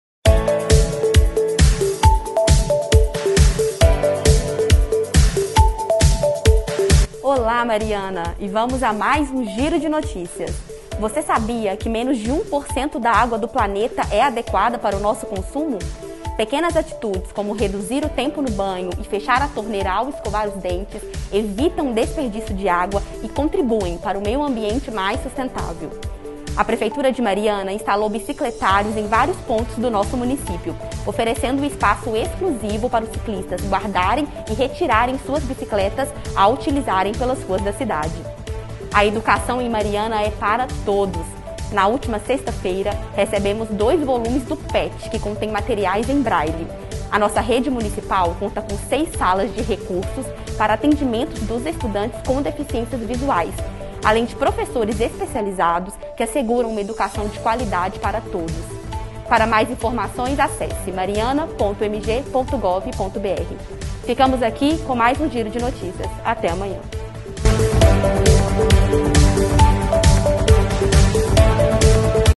Giro de Notícias - 30/06/2021
Confira agora o Giro de Notícias! Fique por dentro das principais notícias da Prefeitura de Mariana.